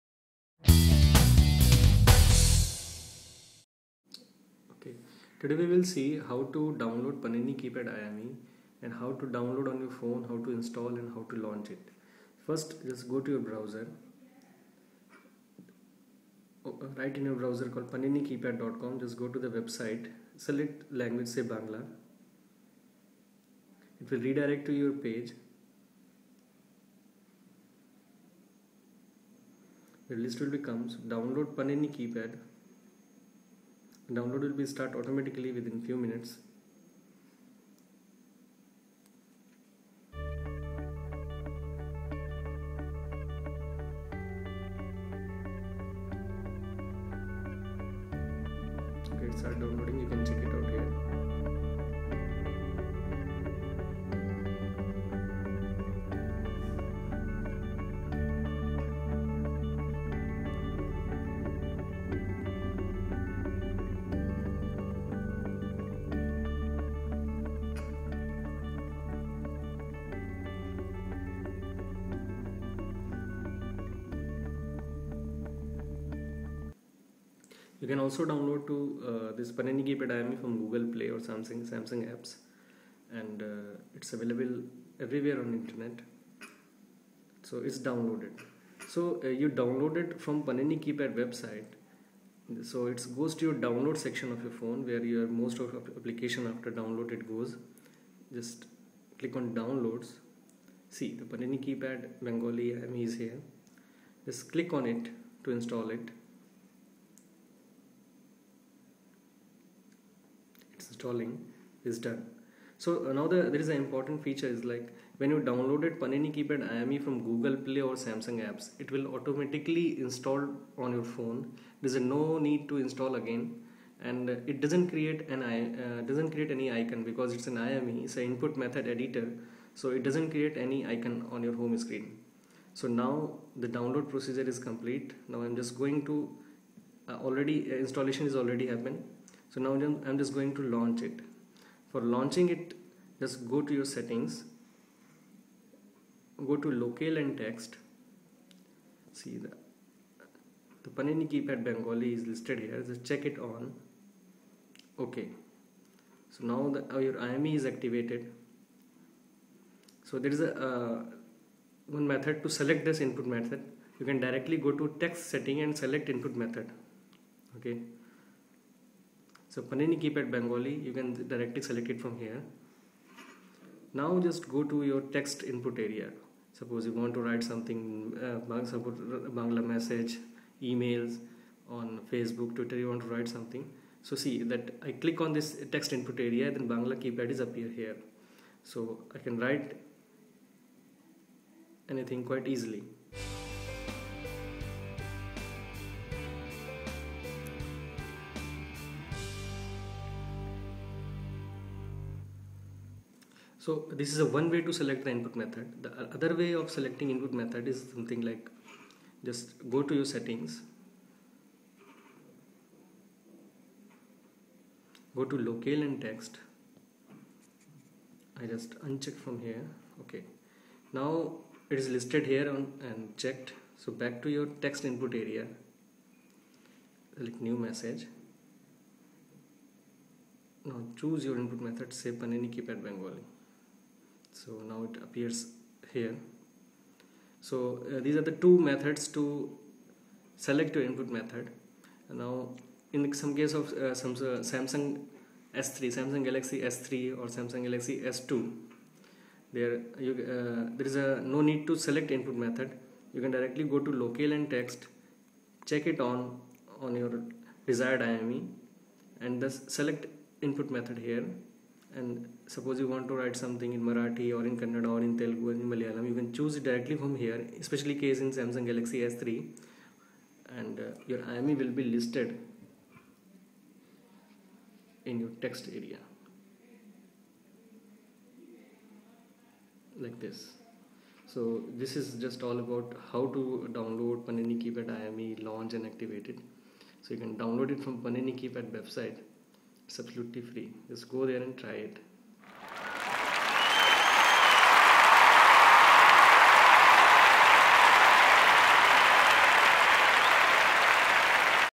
Type in Bengali/Bangla on phone using Panini Keyboard IME Installation
Typing in Bangla/ Bengali bhasha never been easier on phones and other digital devices. we all have some traditional fixed keypad or transliterated method to write in Indian script.
But now there is  a new innovative technology PaniniKeypad that allows users to type in Bengali /Bangla on phones and tablets quite fast & quite easily. Panini Keyboard works on statistical predictive texting. so every time when user type one character , next character will appear automatically on screen. That helps users to type quite fast in Bangla on phone.

PaniniKeypad for android is an IME (input method editor), its installation & launch process is little different from software or app. After installation this doesn't create any icon on home screen. you have to follow some settings to launch this IME.
The biggest benifit of PaniniKeypad is that you can type in Bangla any where on your phone like sms, messages, chat, twitter, update status on facebook, type kavita, lyricrs etc.

Also available on Samsung Apps .

Installation:
1. Download and install this application on phone.
2. Go to "Setting"-"Locale & text" and tick in check box on Panini Keypad Bengali.
3. Go to any text filed in which you want to type.
4. Long press on text field and choose "Input Method".
5. Select this IME (Panini Keypad Bengali)

It is noticed that in latest samsung android phones like Galaxy S2,S3, note 1, note 2, etc.
Go to Settings- Language & input- select PaniniKeypad Bengali IME. (check on)
Then select default keyboard as PaniniKeypad Bengali IME.

Back to your text input area the Bengali keypad will appear on screen.

Enjoy the Bangla typing with PaniniKeyboard.